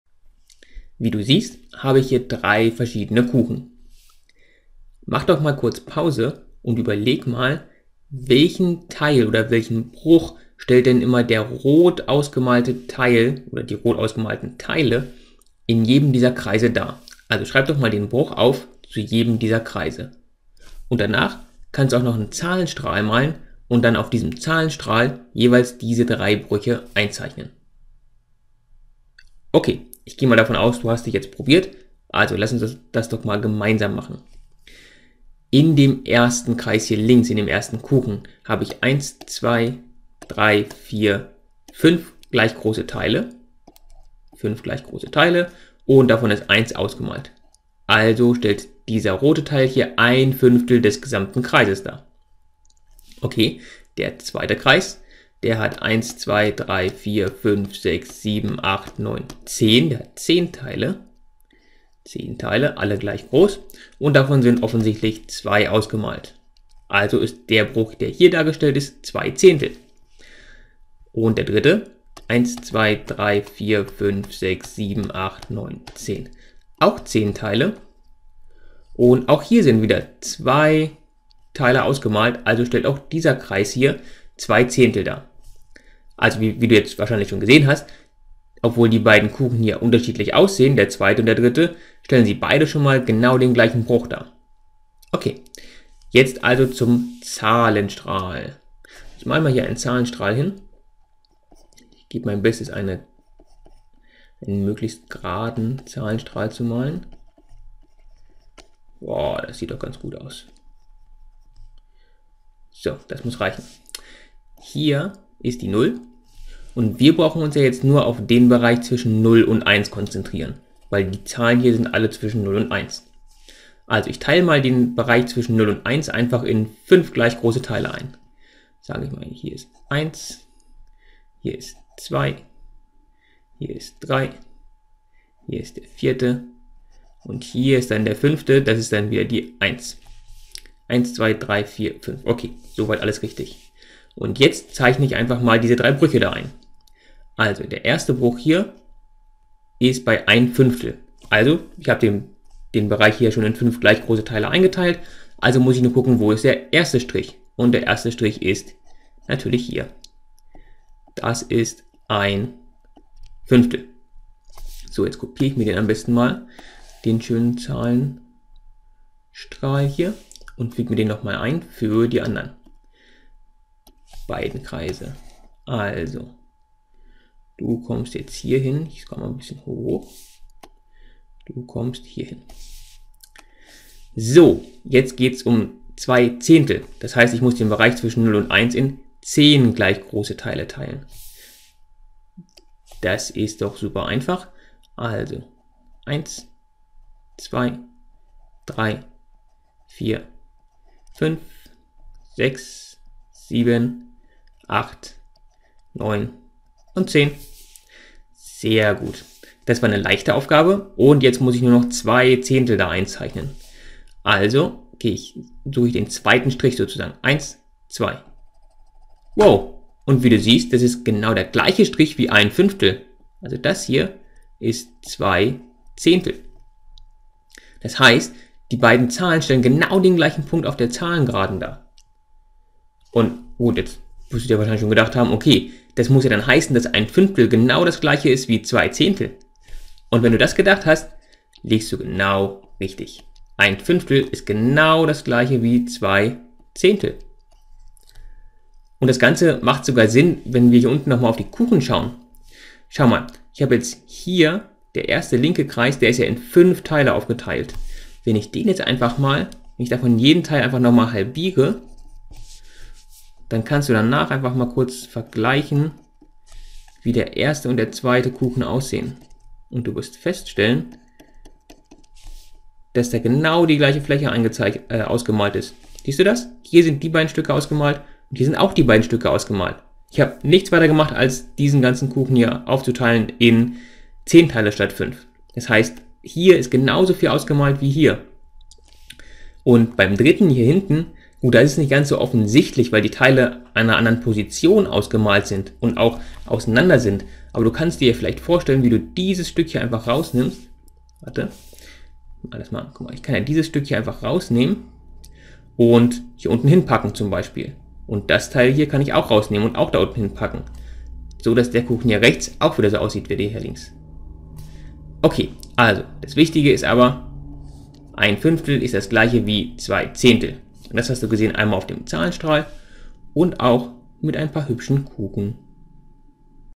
Gleichwertige Brüche visuell und auf einer Zahlengerade
Wie kannst du gleichwertige Brüche bildlich darstellen? Wie liegen gleichwertige Brüche eigentlich zueinander auf dem Zahlenstrahl?Finde es heraus!

Dieses Video basiert auf den englischen Inhalten der Khan Academy und wurde durch Andy Petsch ins Deutsche übersetzt.